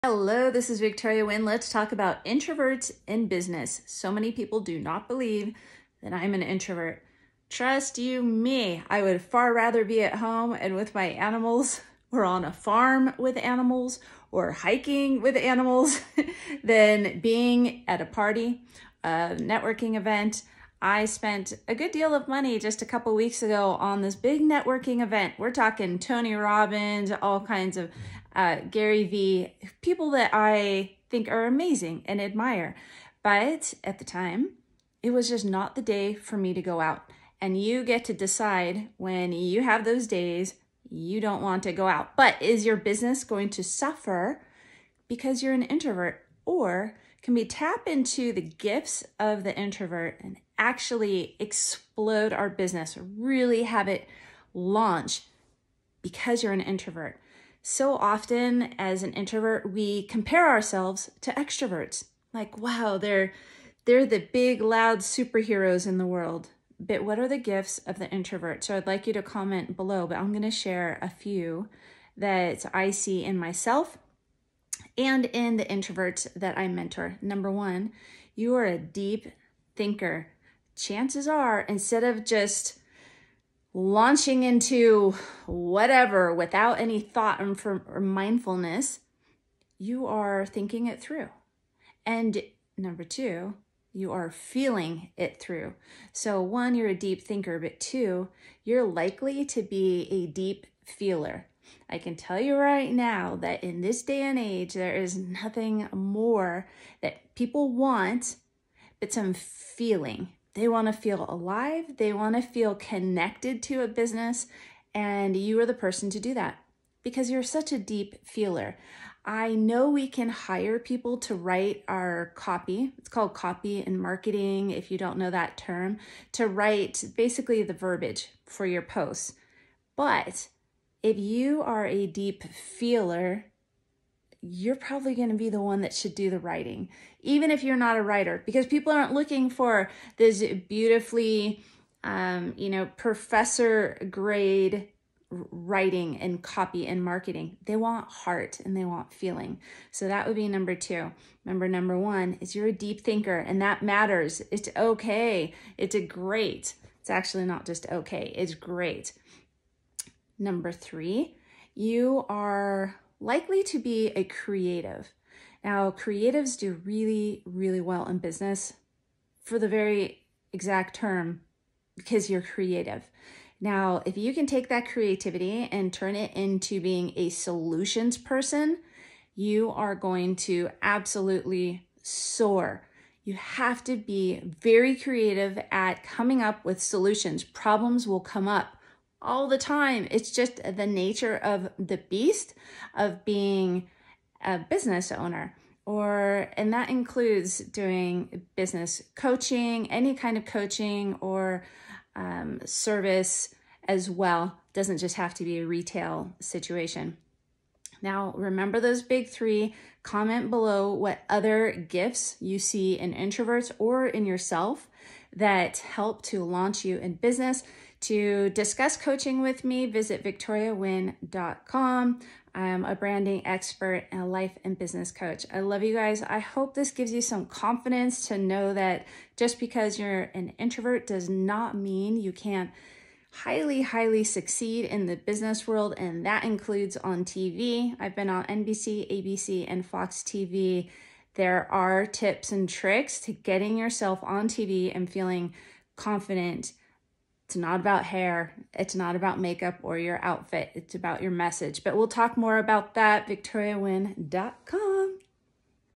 INTROVERT? You May Be Better At Business (and LIFE) Than You Think
I'm what I call an introverted people lover. I'm sure many of you can relate. You truly to care about people, animals and the world around but you relish in alone time and staring at a wall or a book or a piece of art for a while doesn't seem weird to you.  lol

Gifts Of The Introvert:
1. You’re a deep thinker, you’re less likely to do “dumb” things without thought put into it
2. You’re a deep feeling, something the world wants more of right now. We are tired of being numb, introverts breathe love and life into business.
3. You’re creative. Funnel that creativity into becoming a solution finding master! Troubles arise in business on the daily, stay calm and hone your creativity into being great at finding solutions

To work with me as your personal business coach, visit VictoriaWynn.com